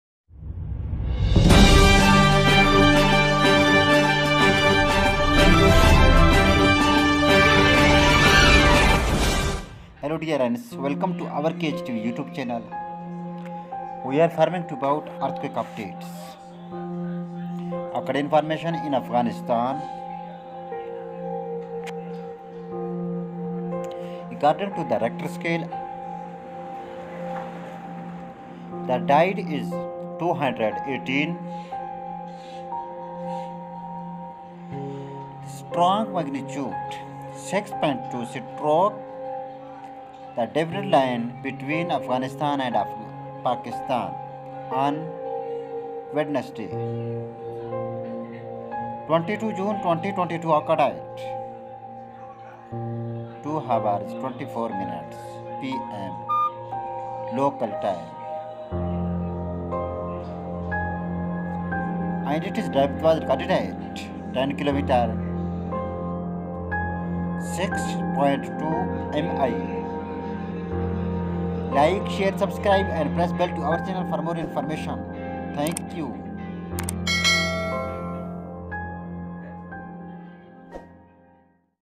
deadly magnitude hits in Afghanistan footage today || Afghanistan earth quake update || @kh tv
kh tv @khtv8943
An earthquake measuring moment magnitude (Mw ) 6.2 struck the Durand Line, between Afghanistan and Pakistan on Wednesday, 22 June 2022 at 02:24 Afghanistan Time (UTC+4:30). The earthquake destroyed hundreds of houses in Afghanistan. According to the United States Geological Survey, the earthquake measured Mwb  5.9 and occurred at a depth of 10 km (6.2 mi).[1] It was felt in the southern parts of India and Pakistan's capital Islamabad and eastern Punjab province.[2] At least 280 people died and 600 were injured.[3][4]

 2022 Afghanistan-Pakistan earthquake
 UTC time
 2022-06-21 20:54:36
 USGS-ANSS
 ComCat
 Local date
 June 22, 2022
 Local time
 02:24 (UTC+4:30)
 Magnitude
 6.2 Mw
 5.9 Mwb
 Depth
 10.0 km (6.2 mi)
 Epicenter
 33.092°N 69.514°E
 Type
 strike-slip
 Max. intensity
 VII (Very strong)
 Casualties
 280 dead, 600 injured